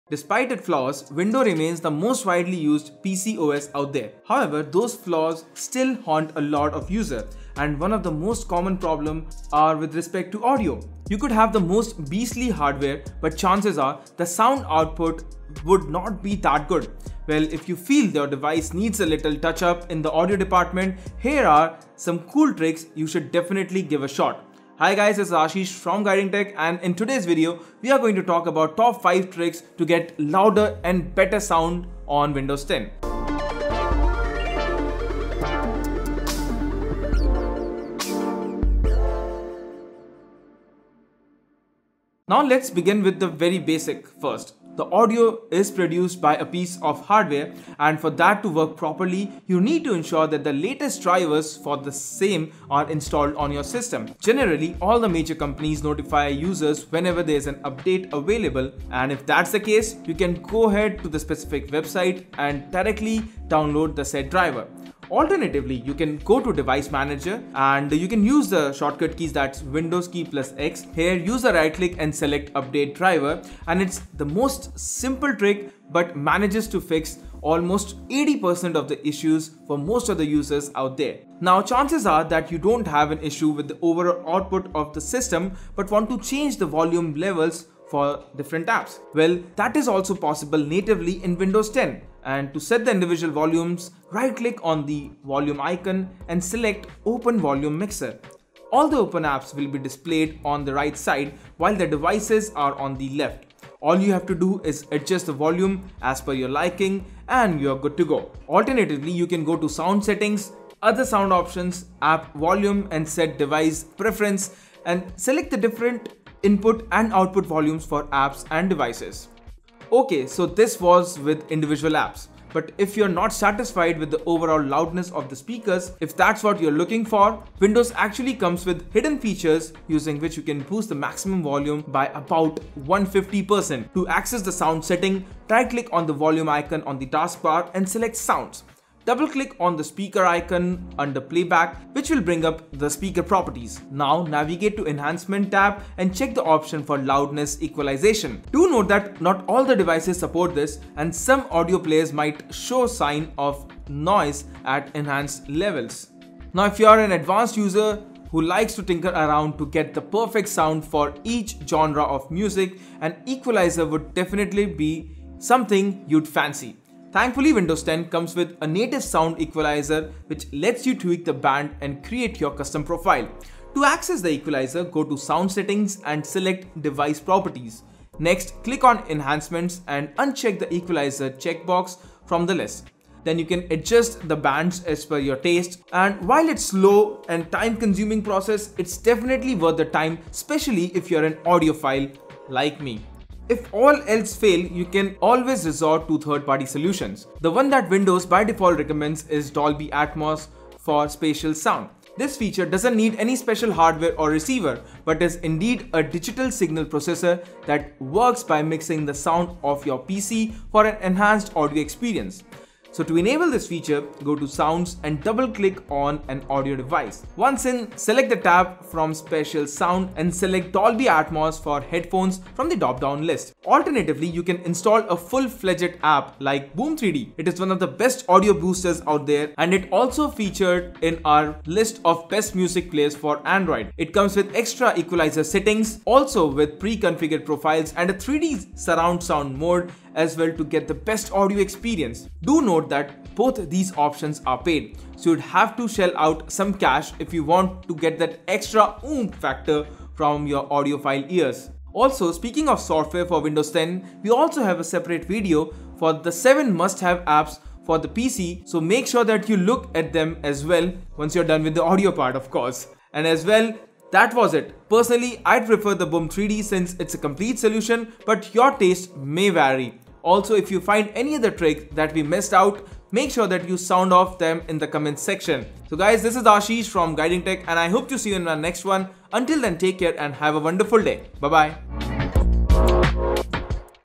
5 Best Ways to Get Louder and Better Sound on Windows 10 | Guiding Tech
Despite its flaws, Windows remains the most widely used PC OS out there. However, those flaws still haunt a lot of users, and one of the most common problems are with respect to audio. You could have the most beastly hardware, but chances are, the sound output would not be that good. Well, if you feel your device needs a little touch-up in the audio department, here are 5 tricks you should definitely give a shot.

APP LINKS

Boom 3D (Trial)